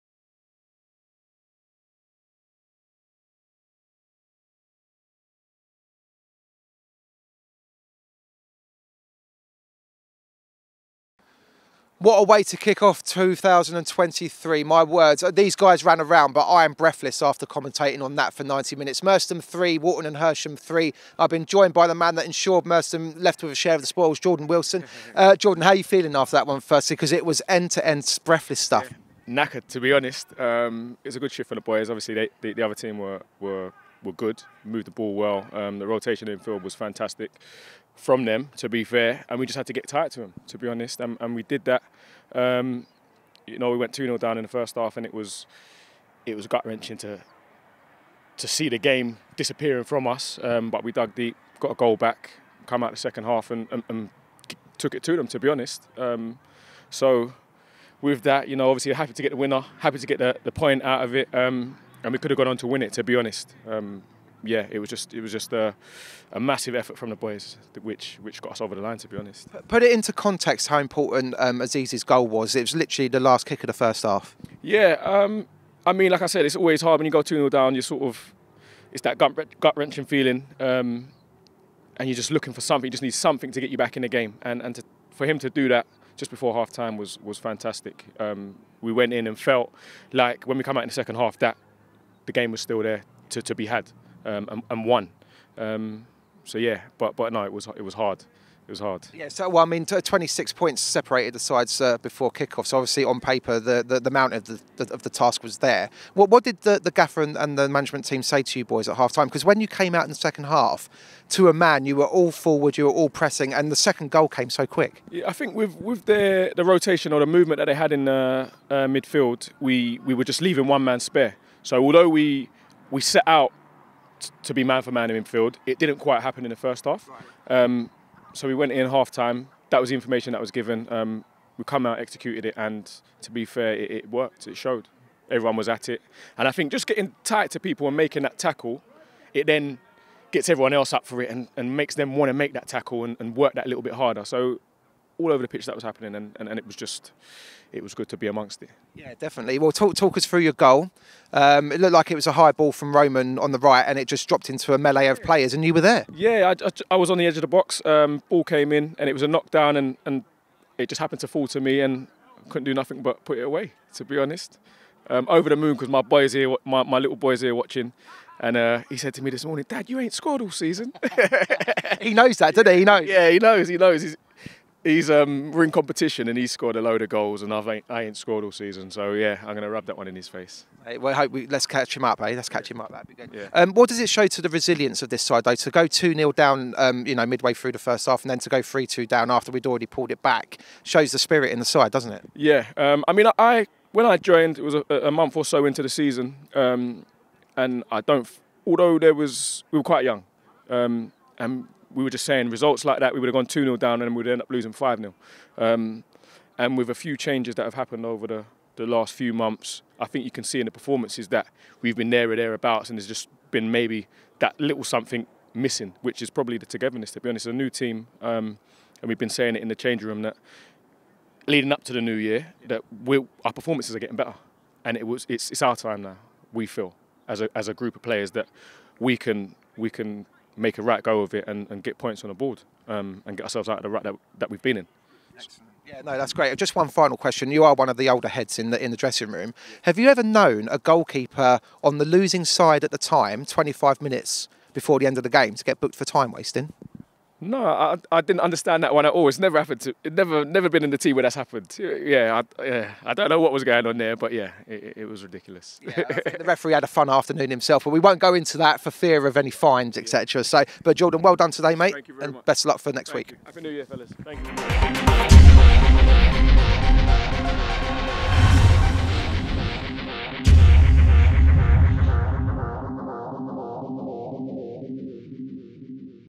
Merstham 3 3 Walton & Hersham | Jordan Wilson Interview | 2nd January 2023
Catch the thoughts of Jordan Wilson when he spoke to James Barrett-Sterling after our home match vs Walton & Hersham.